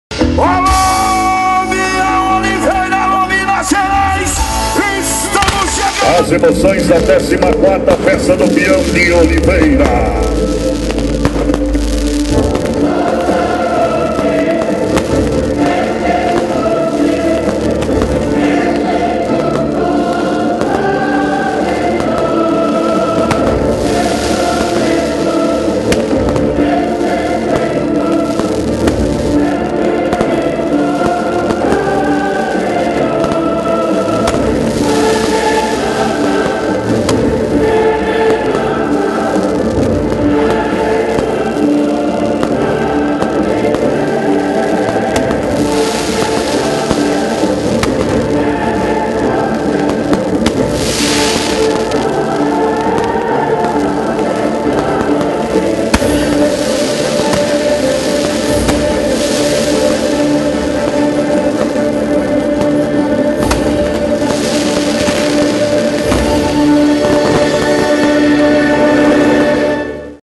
Tim Soier Festa do Peão de Oliveira
Clipe Tim Soier Promoções. Festa do Peão de Oliveira. Ano 2013.
Imagens: Galdino Almeida